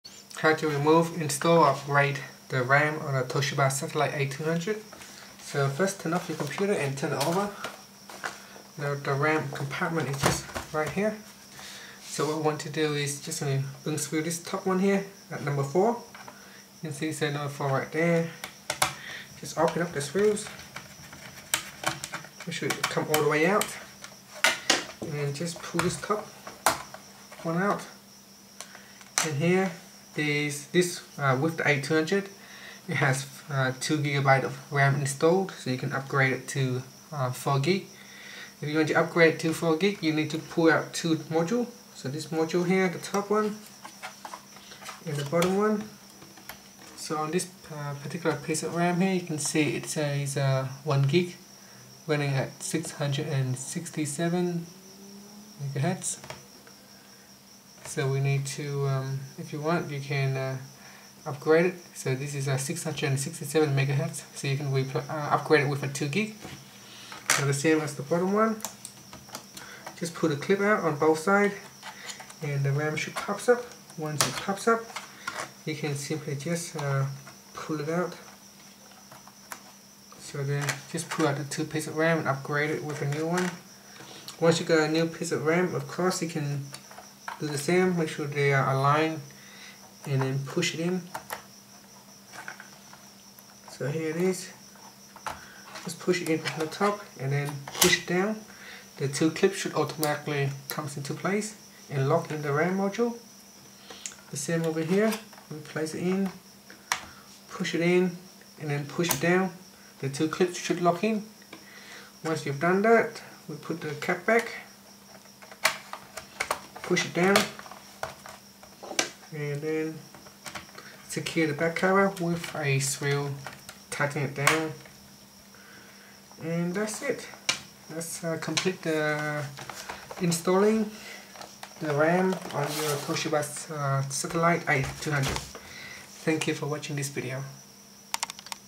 Toshiba Satellite A200: How to Remove and Install Memory RAM Module
Learn how you can remove and install memory RAM module on Toshiba Satellite A200.